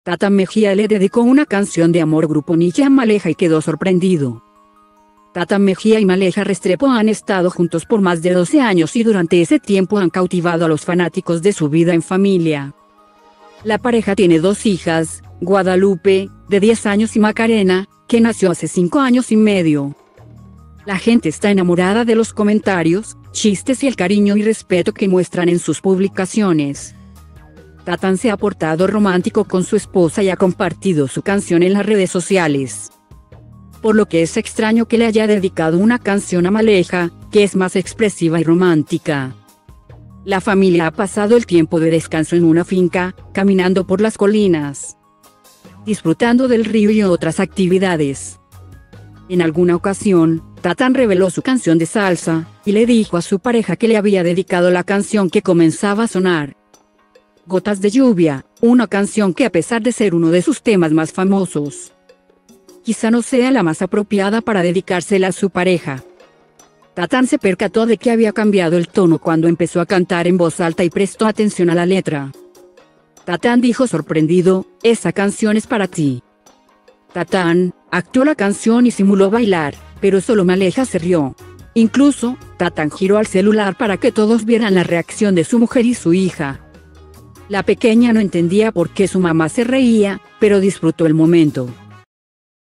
Tatán Mejía le dedicó una canción de amor Grupo Niche a Maleja y quedó sorprendido
Tatán Mejía y Maleja Restrepo han estado juntos por más de 12 años y durante ese tiempo han cautivado a los fanáticos de su vida en familia.La pareja tiene dos hijas, Guadalupe, de 10 años y Macarena, que nació hace cinco años y medio. La gente está enamorada de los comentarios, chistes y el cariño y respeto que muestran en sus publicaciones.